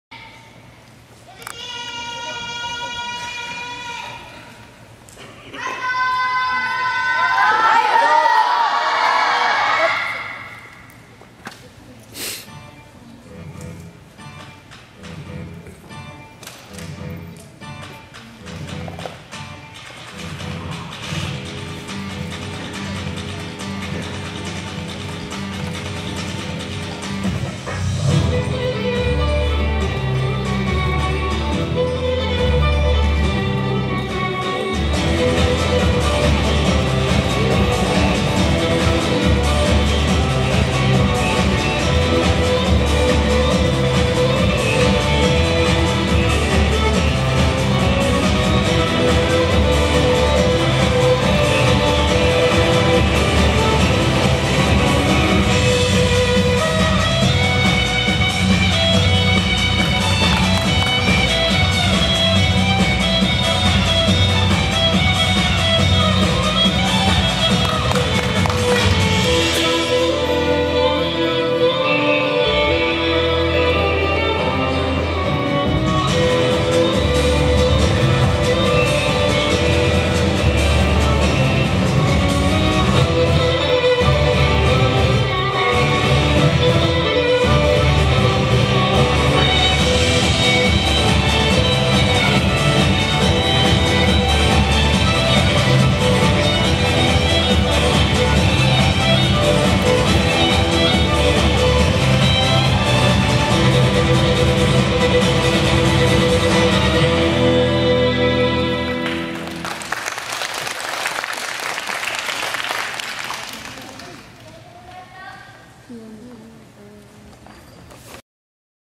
SanixCup08中島柚輝・徒手
2019/2/10 SanixCup2019　中島柚輝(小２）
　タンブは強く、スピード感満載で驚きの迫力がある。上下肢でグンともう一伸び見せる心意気がとてもいい、背倒立の足の締りは細かな意識の徹底が感じ取れ、跳躍の柔軟さや胸張る大きなたわみはとても美しい